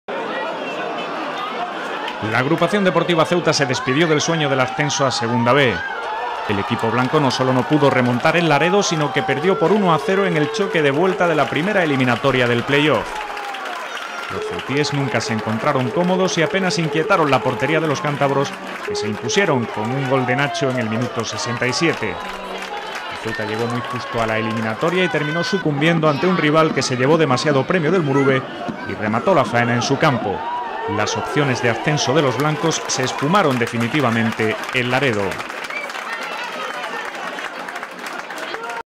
Laredo 1 - Ceuta 0 (25-05-14)
Resumen del partido Laredo 1 - Ceuta 0 de los 1/4 de final del playoff de ascenso a Segunda B de la temporada 2013-14.